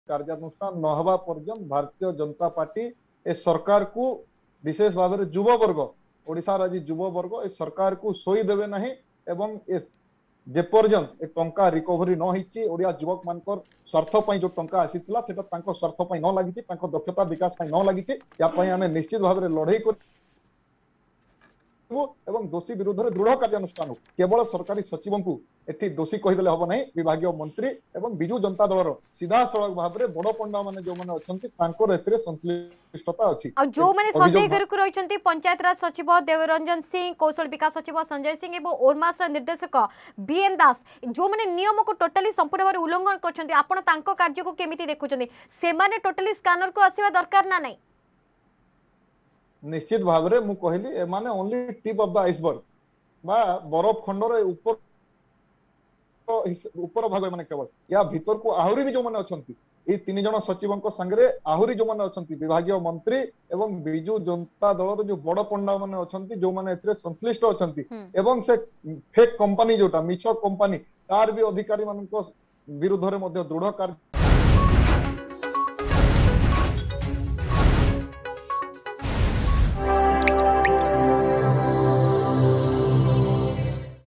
State government should invest the money in the development of the youth
ଯୁବକମାନଙ୍କ ବିକାଶ ପାଇଁ ଆସିଥିବା ଟଙ୍କାକୁ ସଠିକ୍ କାର୍ଯ୍ୟରେ ଲଗାନ୍ତୁ ରାଜ୍ୟ ସରକାର
ଶୁଣନ୍ତୁ ବିଜେପି ଯୁବ ମୋର୍ଚ୍ଚା ସାଧାରଣ ସମ୍ପାଦକ ଓମ୍ ପ୍ରକାଶ ମିଶ୍ରଙ୍କ ପ୍ରତିକ୍ରିୟା...